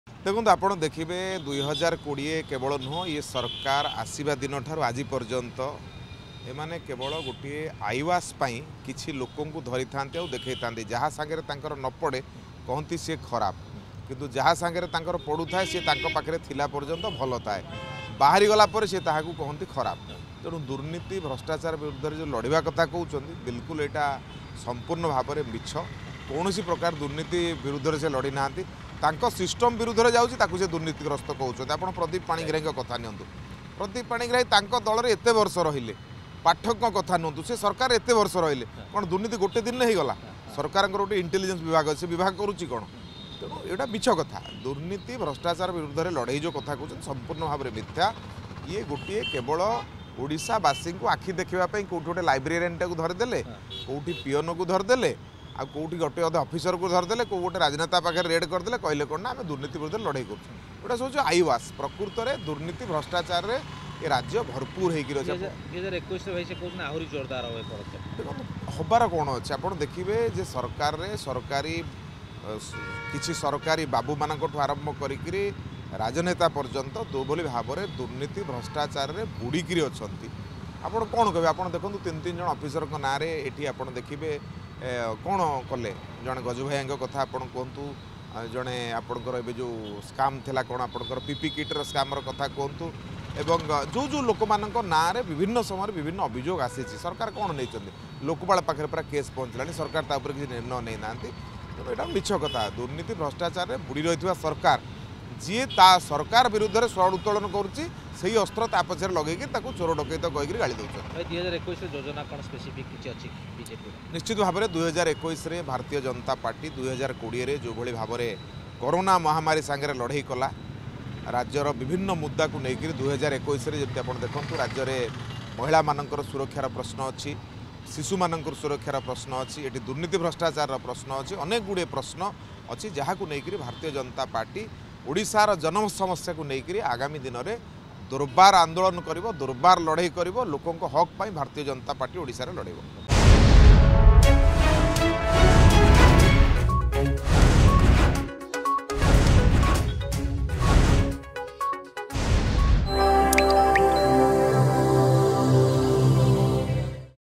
ମୁଖ୍ୟମନ୍ତ୍ରୀଙ୍କ ଦୁର୍ନୀତି ବିରୋଧ ଅଭିଯୋଗ "ଆଇ ଓ୍ବାସ୍ବ"
ମୁଖ୍ୟମନ୍ତ୍ରୀଙ୍କ ଦୁର୍ନୀତି ବିରୋଧ ଅଭିଯୋଗକୁ "ଆଇ ଓ୍ବାସ୍ବ" ବୋଲି କହିଲା ବିଜେପି.